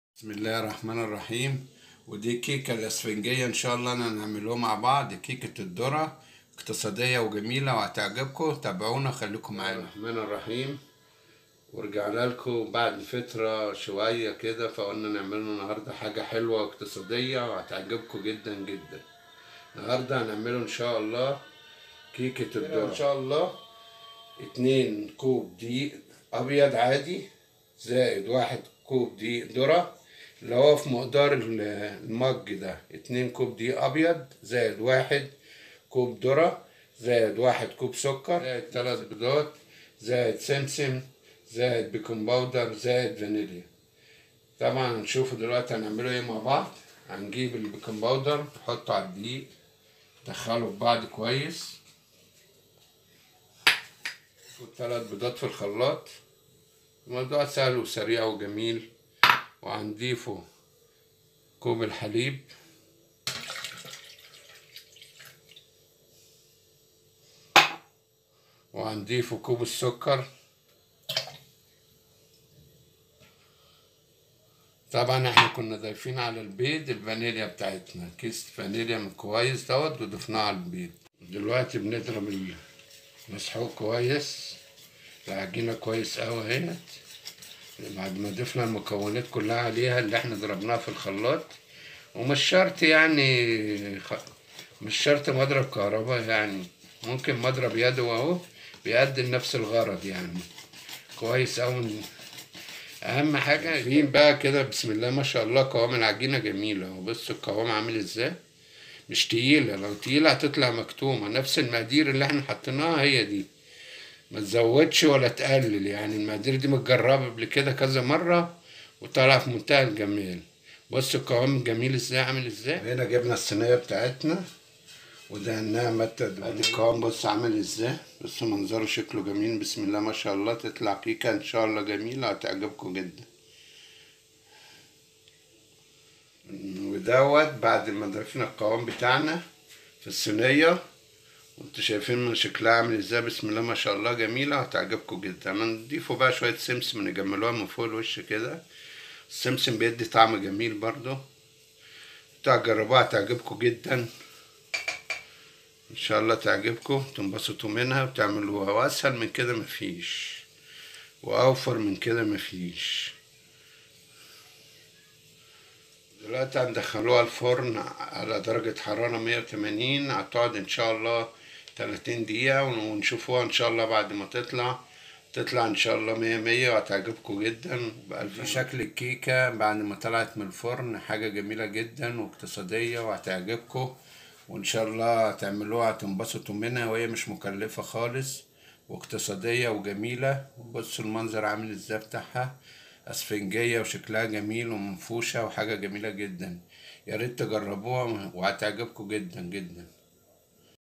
كيكه الذره الاقتصاديه بااقل التكلفه مكونات موجوده في كل بيت
كيك الذره
# اجمل كيك
# كيك الاقتصاديه